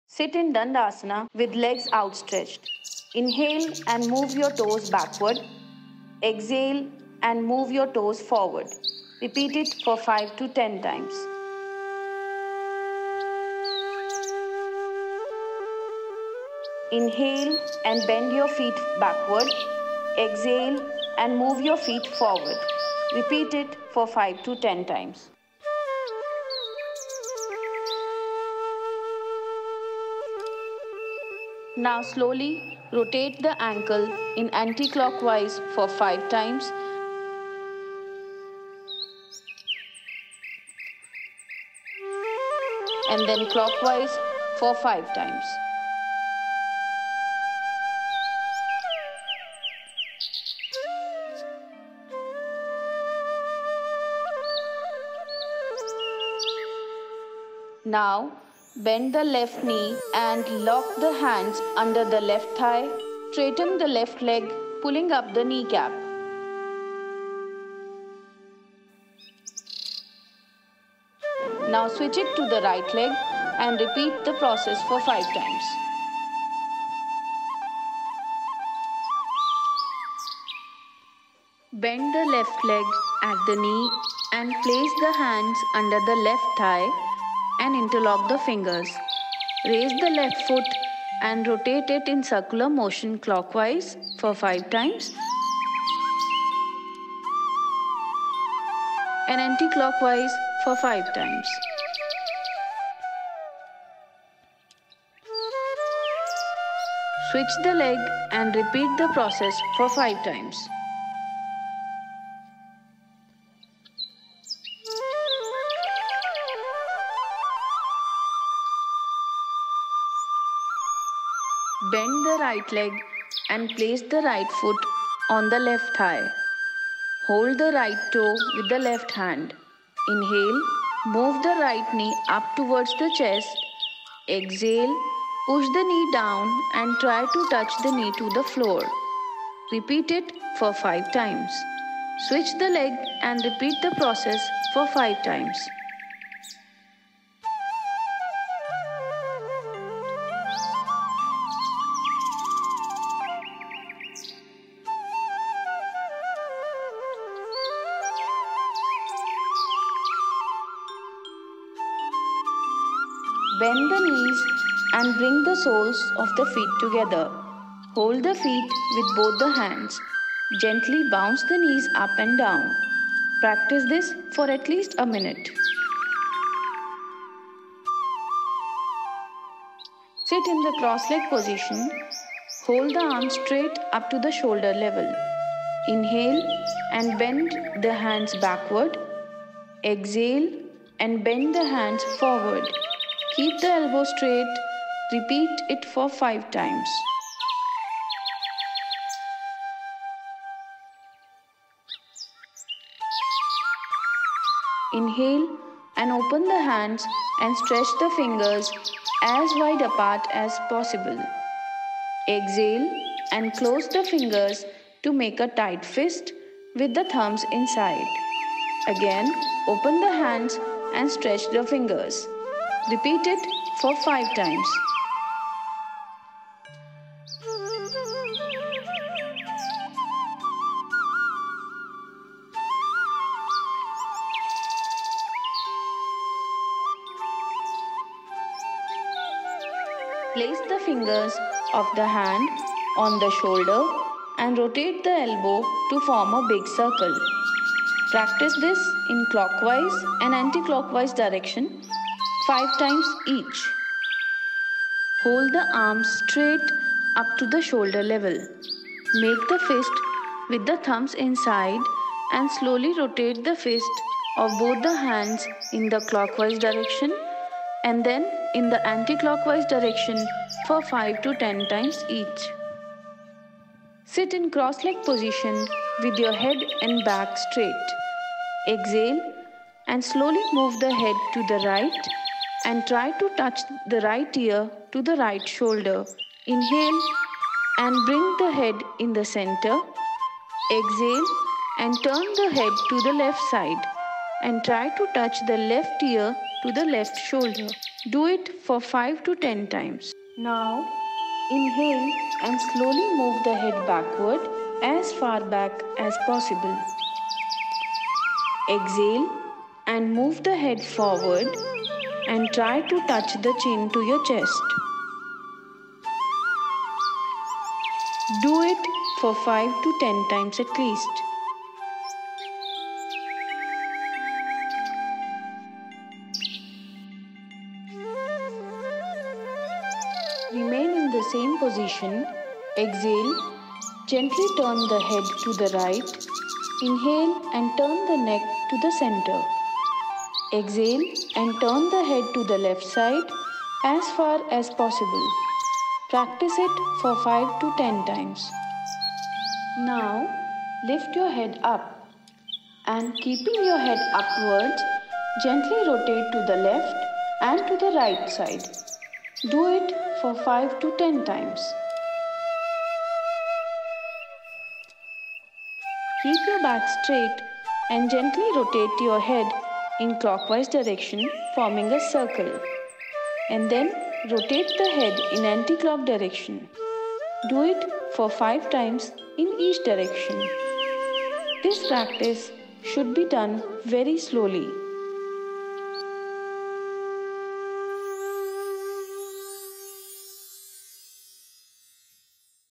Pawanmukt Aasan - Series 1 | Step-by-Step Guide
Welcome to our new playlist dedicated to the transformative practice of Pawanmukt Aasan Series 1! In this comprehensive tutorial, we'll guide you through each posture with precision and clarity, empowering you to harness the countless benefits of this ancient yogic sequence.

Pawanmukt Aasan Series 1, also known as the "Anti-Rheumatic Series," is a gentle yet powerful set of asanas designed to release tension, improve flexibility, and promote overall well-being. Whether you're a seasoned yogi or a beginner exploring the world of yoga, this step-by-step guide is your gateway to a healthier, more balanced life.

Join us as we delve into the nuances of each posture, providing detailed instructions on proper alignment, breathing techniques, and modifications to suit your individual needs. From the gentle movements of the neck and shoulders to the soothing stretches of the ankles and toes, this series offers a holistic approach to mind-body wellness.

Embark on this journey with us, and discover the profound healing potential of Pawanmukt Aasan Series 1. Whether you practice in the comfort of your own home or join us in the studio, let's cultivate inner harmony and vitality together.

Let's embark on this journey toward health and wholeness as a community! 🙏✨